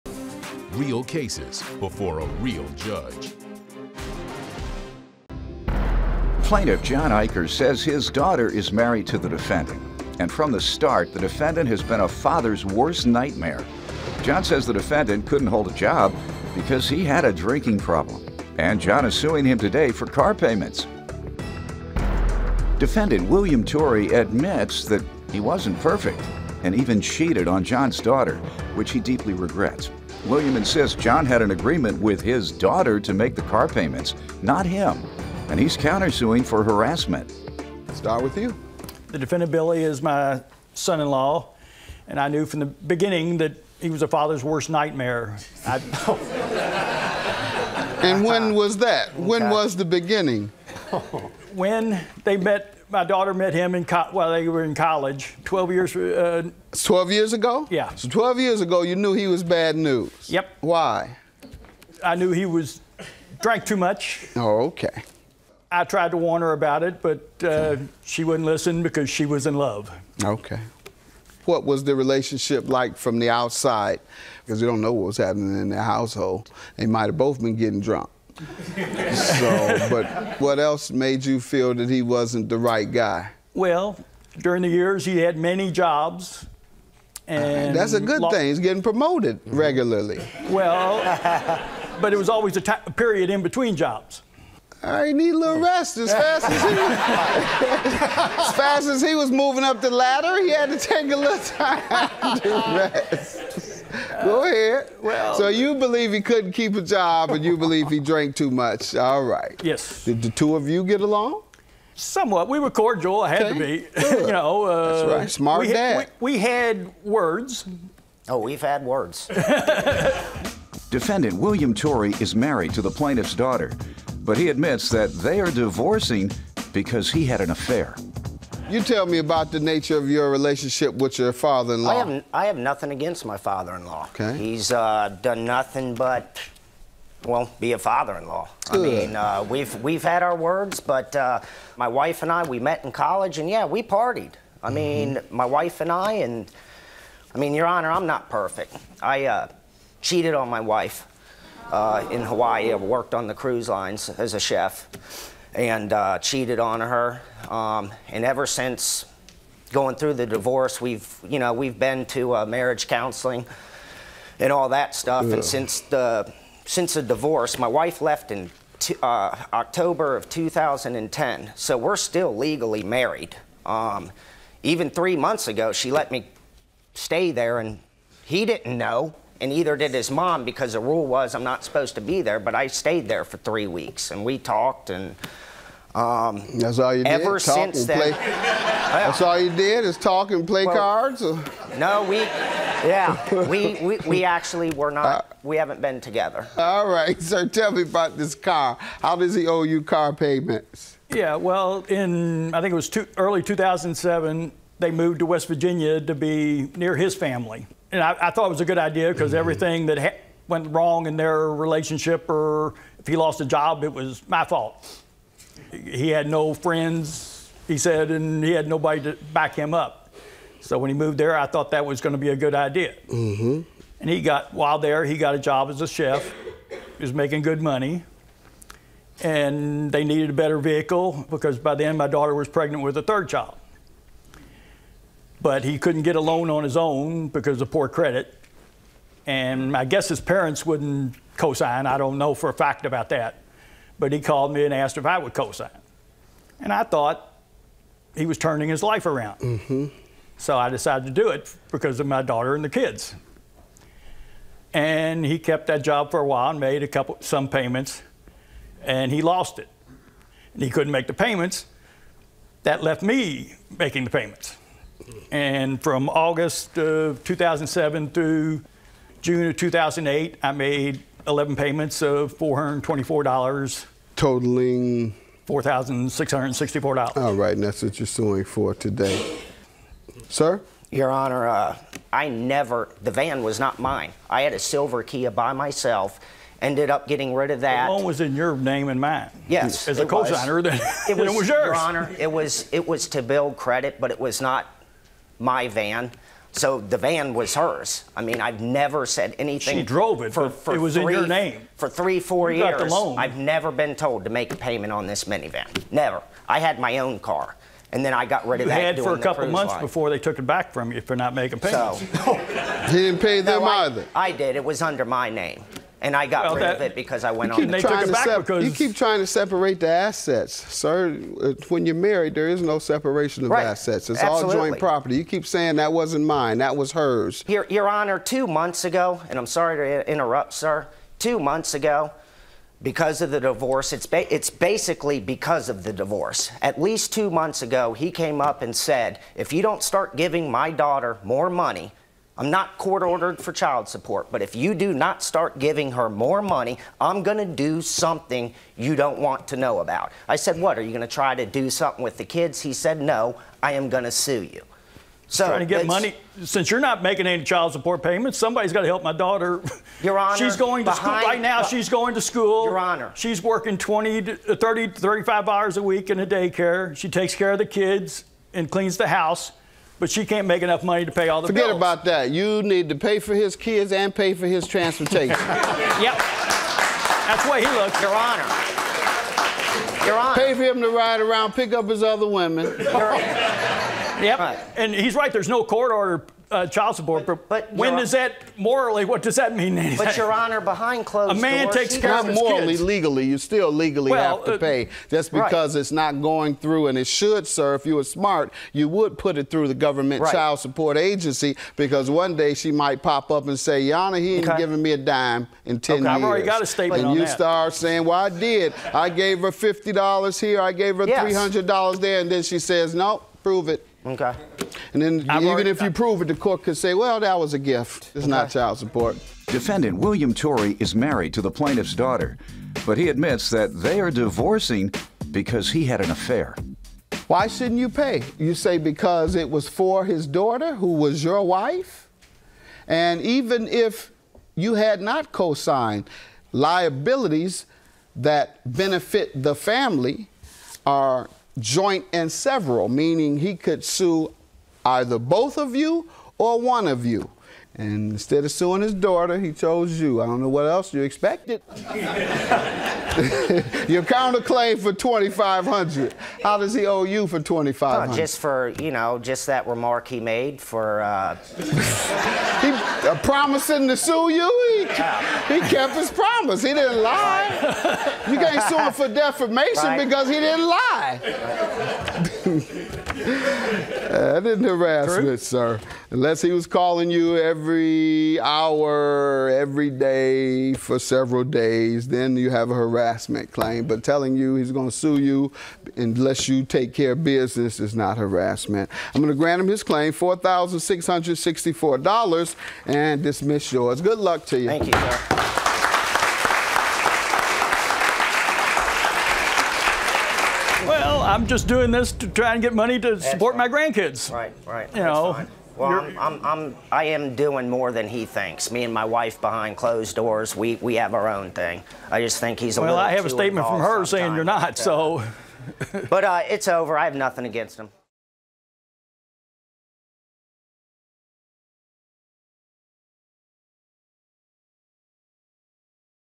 Sour Son-in-law
The plaintiff claims his son-in-law could never hold down a job because of a drinking problem. The plaintiff co-signed on a car loan for the defendant to help him out. However, the defendant lost his job and failed to make the payments, so the plaintiff is suing. The defendant insists the vehicle in question belonged to the plaintiff's daughter, not him.